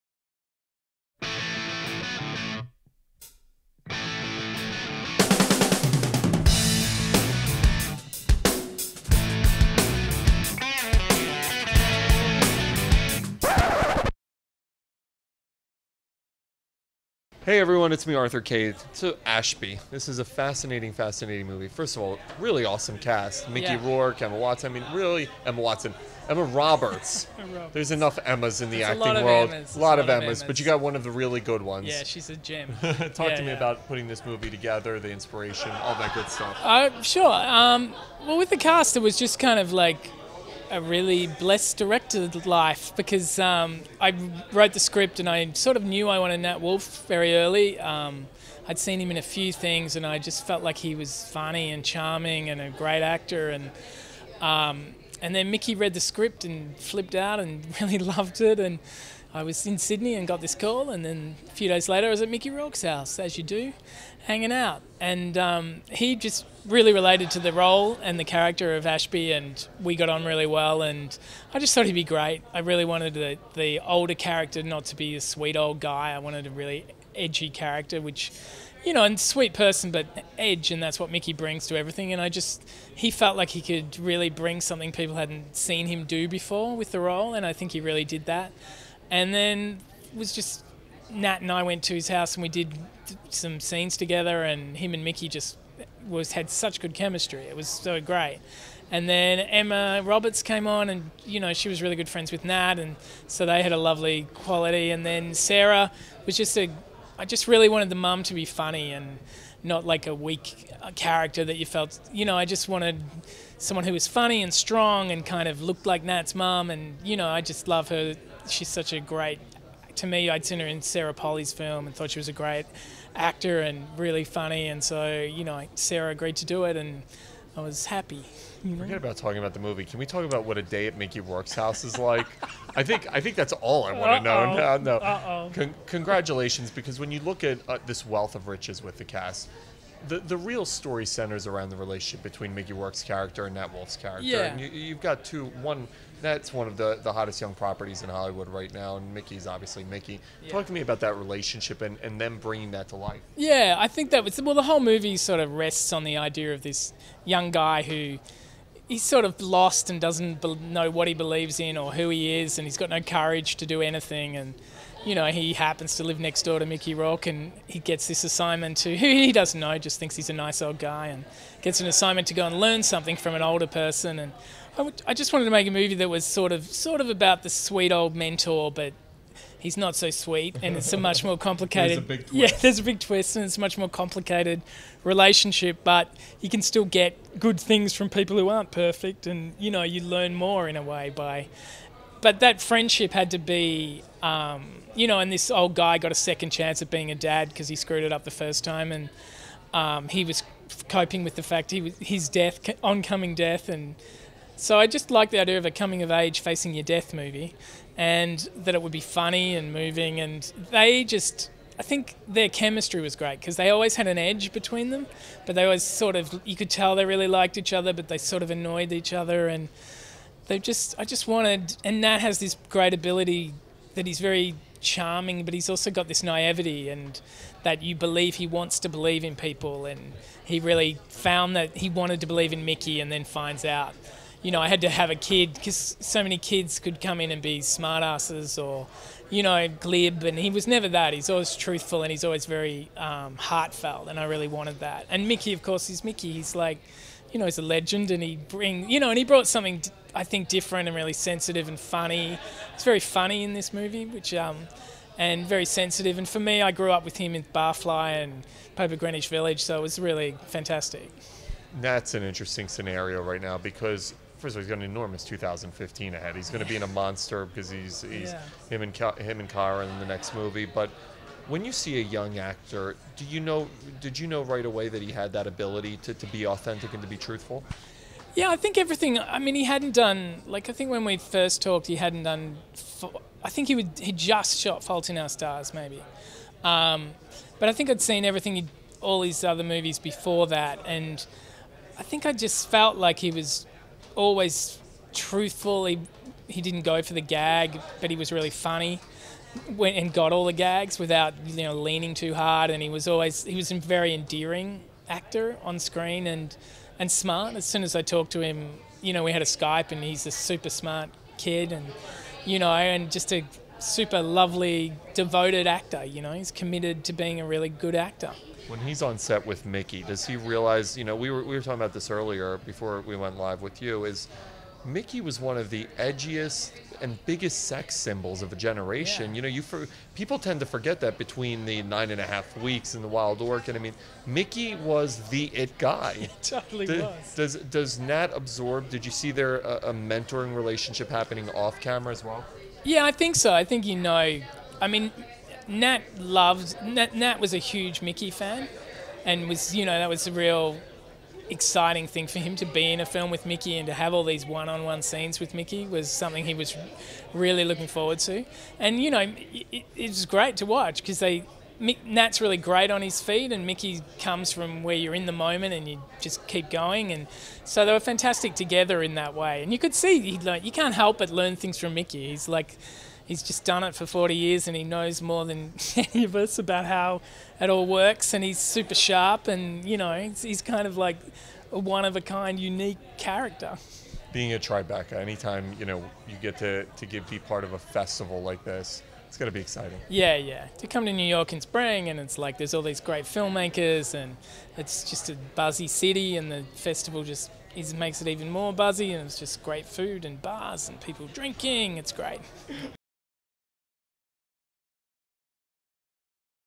Writer/Director Tony McNamara on "Ashby"
Writer/Director Tony McNamara talks about screening his film "Ashby" as part of the 2015 Tribeca Film Festival. The film centers around teenager Ed Wallis (Nat Wolff) who finds out the secret past of his older neighbor (Mickey Rourke.